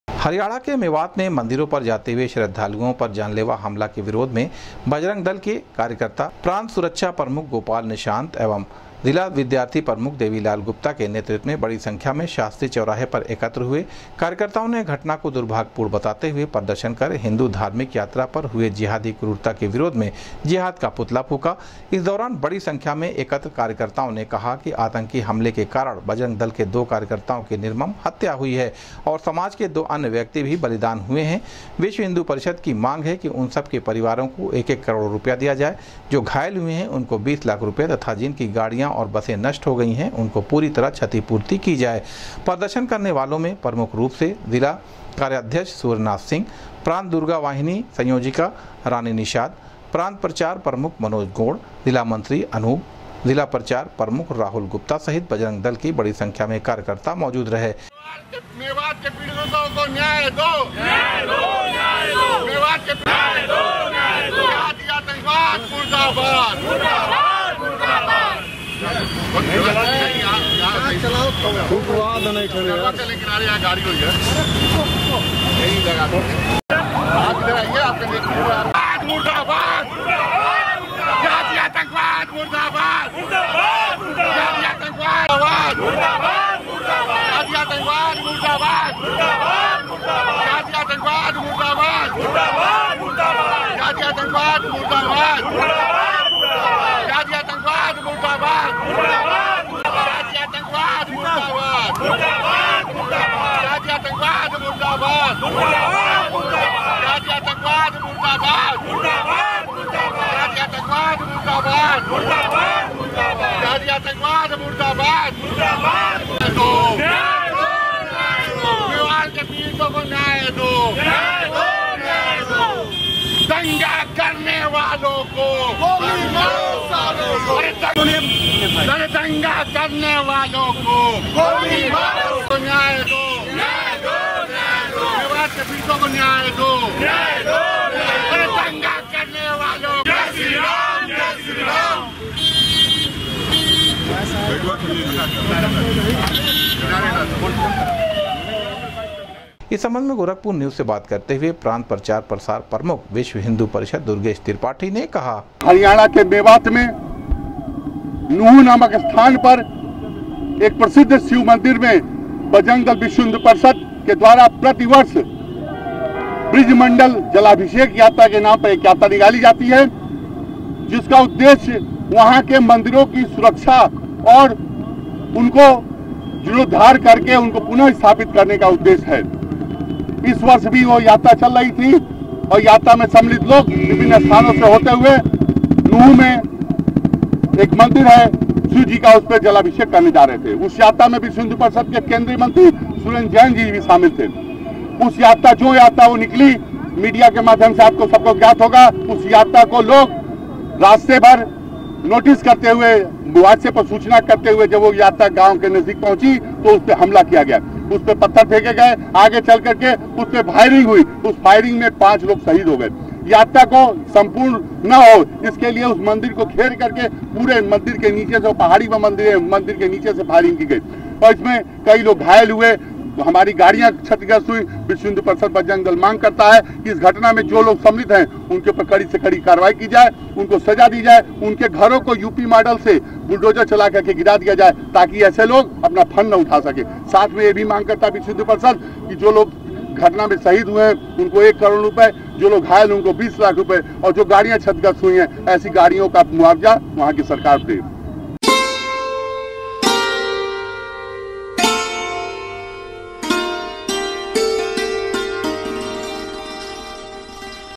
हरियाणा के मेवात में मंदिरों पर जाते हुए श्रद्धालुओं पर जानलेवा हमले के विरोध में बजरंग दल
हरियाणा के मेवात में मंदिरों पर जाते हुए श्रद्धालुओं पर जानलेवा हमले के विरोध में बजरंग दल के कार्यकर्ताओं ने जिहाद का फूंका पुतला, जताया विरोध